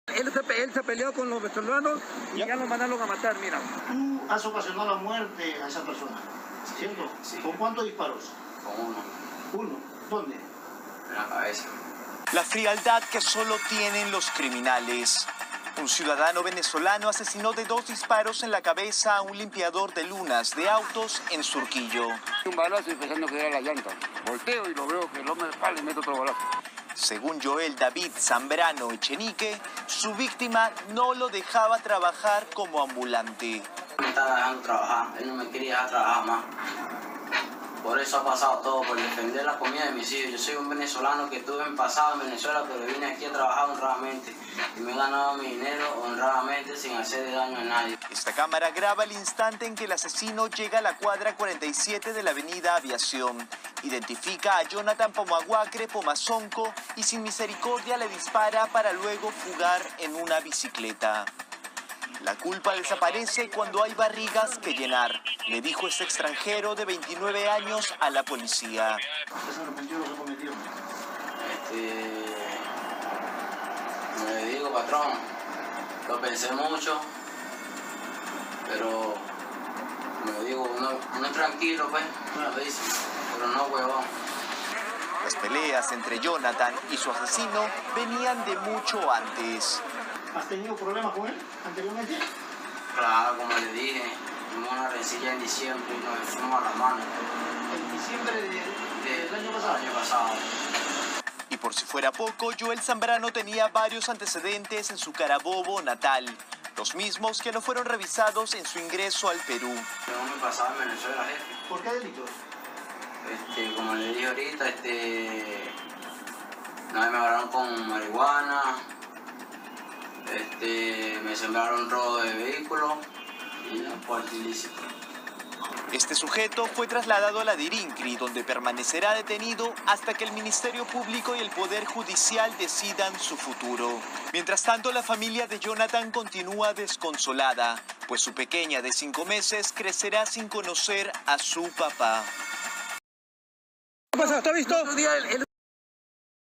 La fría confesión del venezolano que asesinó a su paisano limpiavidrios en Perú
Un ciudadano de nacionalidad venezolana fue detenido tras asesinar de dos balazos en la cabeza a un limpiavidrios de autos, en Surquillo, Perú. El intervenido señaló que cometió el crimen porque la víctima no lo dejaba trabajar en la vía pública.